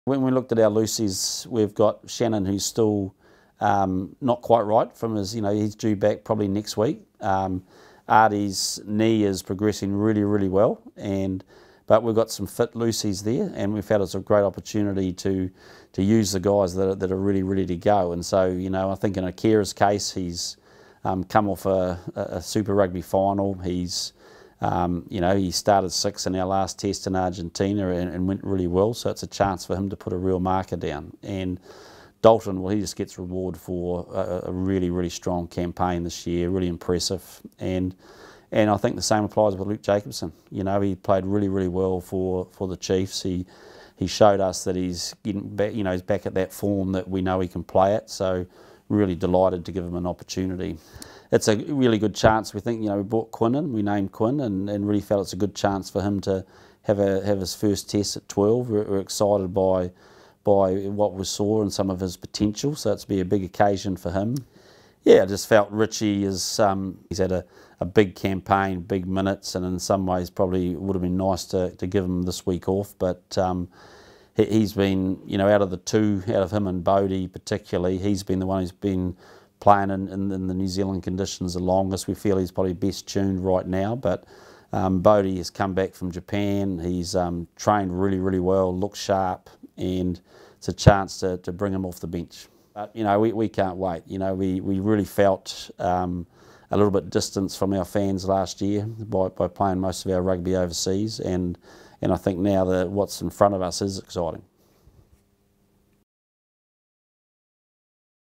Team Naming | Ian Foster discusses first team of 2021
All Blacks coach Ian Foster chats through his key selections for the test against Tonga at Mt Smart Stadium on Saturday.